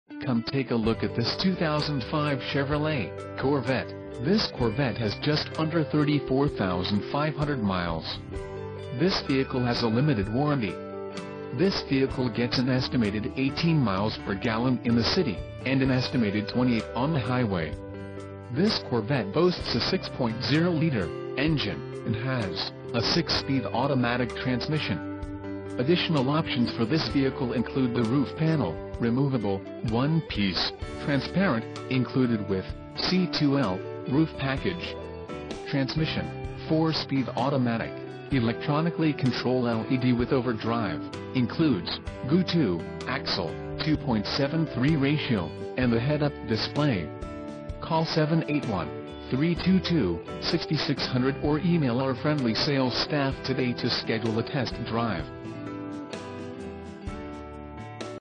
2005 Chevrolet Corvette 2dr Cpe Coupe - malden, MA
Richards Motorcars, malden, MA - Pristine Condition / Chrome Z06 Wheels / Navigation / Priced To Sell!